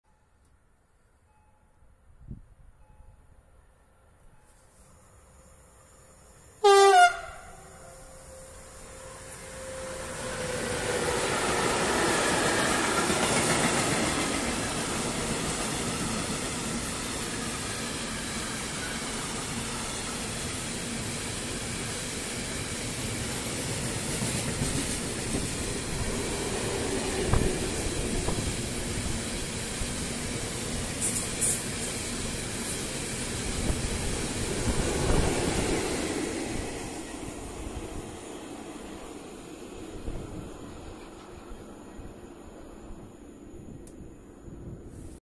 193-738+SM42-2489 ze składem cystern+Rp1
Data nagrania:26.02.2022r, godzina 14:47

Lokalizacja:p.o. Grzęska

193-738+SM42-2489 prowadzą skład cystern w stronę Rzeszowa.

Pozdrowienia dla Pana Maszynisty! :)